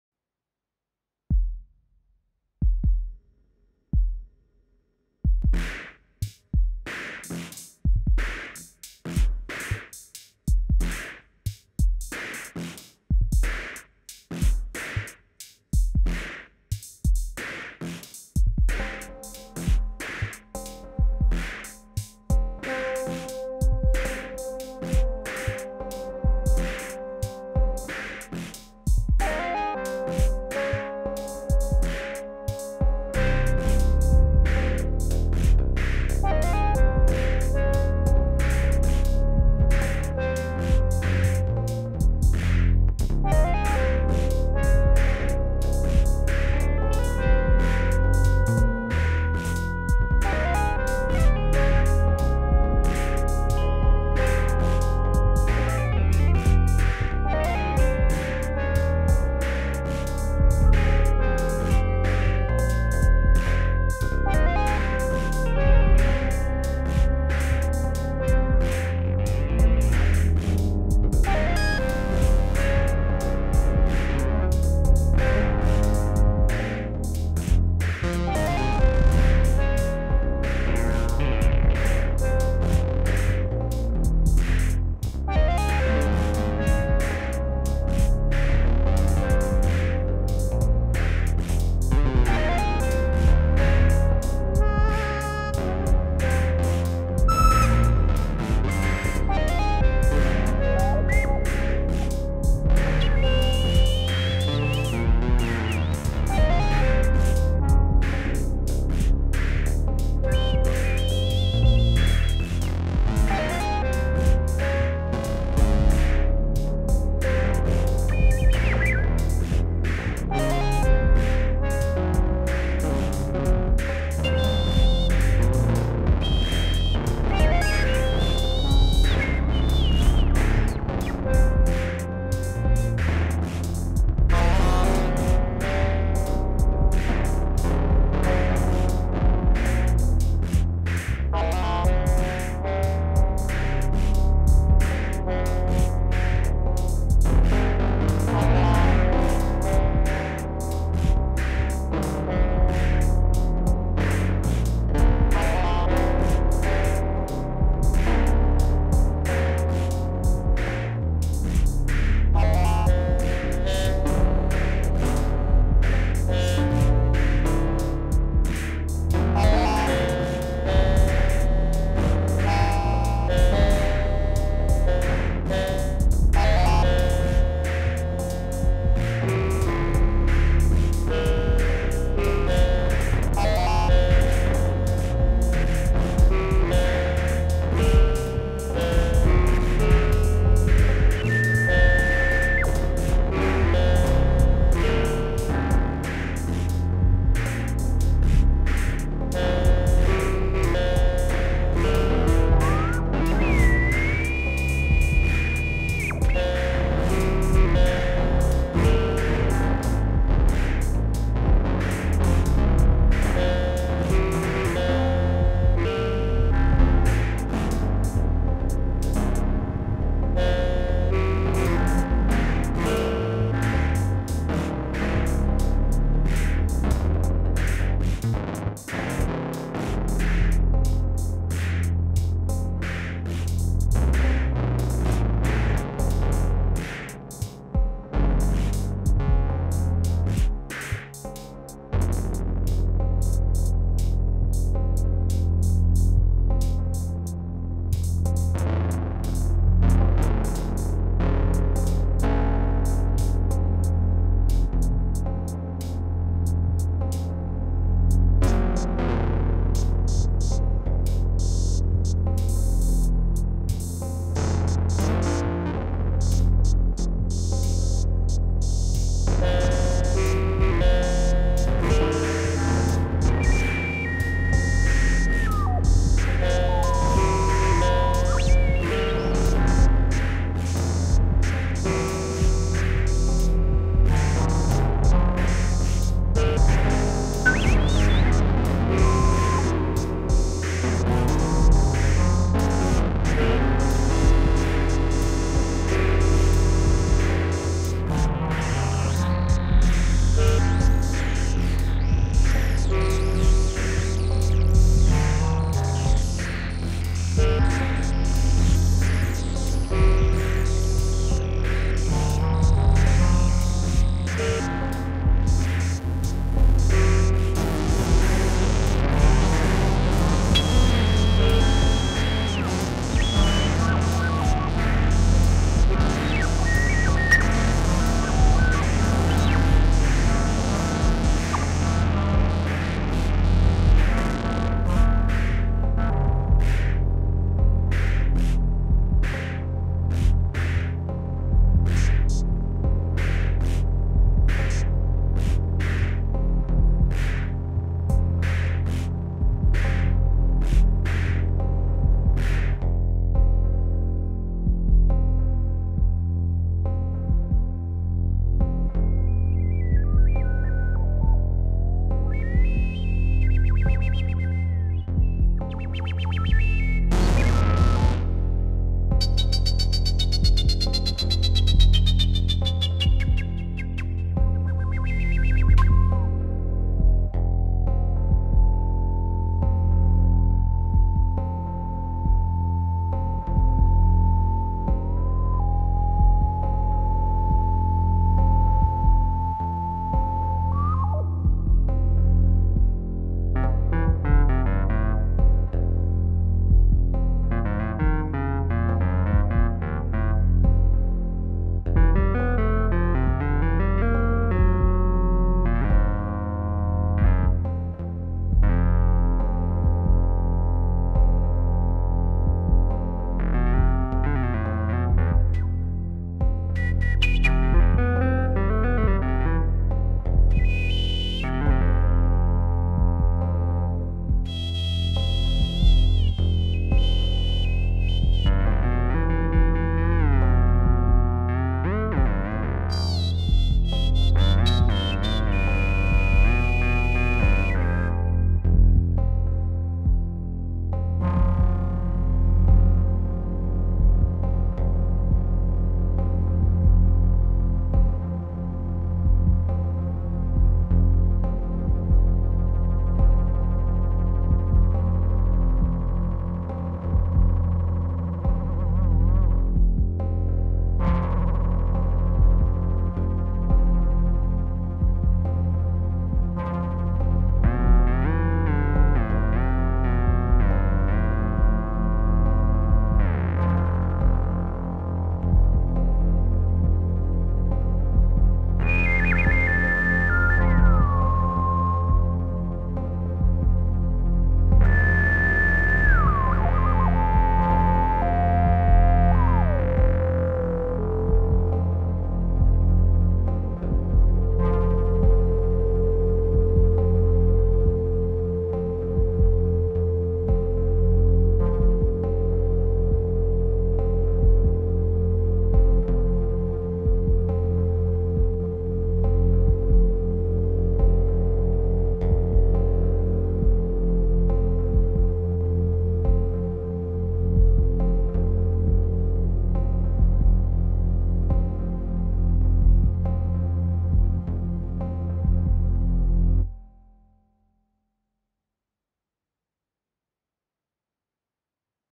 Behringer, Arturia & Continuumini Synth Jam #3
Improvised Free Punk Jazz finishing in Free Monk Ambient Jam using the Behringer Odyssey, Model D and Neutron, the Arturia Beatstep Pro and Drumbrute Impact (with a few effect pedals), and the Continuumini by Haken.